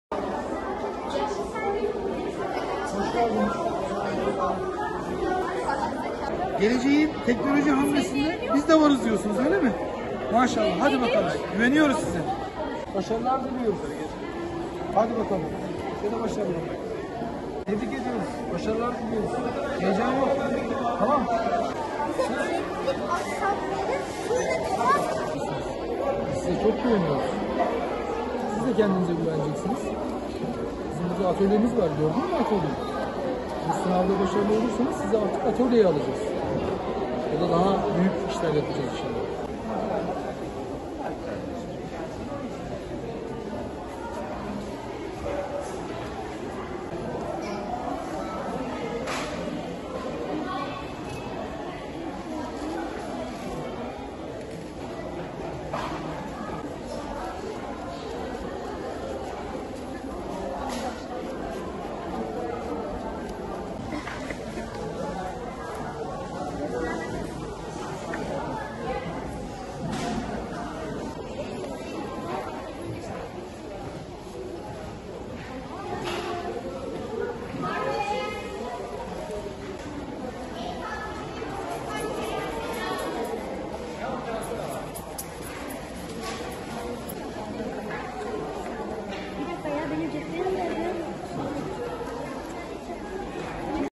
DENEYAP Atölyesi'ne girebilmek için ter döktüler
Düzce'de geleceğin mühendislerini yetiştirecek olan DENEYAP Atölyesi'ne girebilmek için 425 öğrenci ter döktü.

Düzce Gençlik Merkezi'nde gerçekleştirilen DENEYAP Atölyesi uygulama sınavına, Düzce’de 1-26 Haziran tarihleri arasında toplam bin 175 öğrencinin katılımı ile yapılan e-sınavda başarılı olan 425 öğrenci katıldı. DENEYAP Atölyelerinin öğrenci seçme sınav sürecinde birinci aşamayı tamamlayarak ikinci aşama sınavına hak kazanan adaylar, bu sınavda kendilerine sunulan tema doğrultusunda verilen malzeme kiti ile özgün fikirlerini ortaya çıkaran, teknik bilgilerini kullanabilecekleri proje tasarımı yaptı.
Bu yılki teması “Çevre Dostu Yük Gemisi” olarak belirlenen uygulama sınavında adaylardan verilen sürede tasarladıkları geminin belirlenen hedefe yük taşımasını sağlaması istendi. 2 saatlik süre sonunda tamamlanan projeler belirli kriterler doğrultusunda değerlendirilmek üzere jüri üyelerine sunuldu.
Düzce Gençlik ve Spor İl Müdürü İsa Yazıcı, uygulama sınavı öncesi öğrencilerle yakından ilgilenerek, başarılar diledi.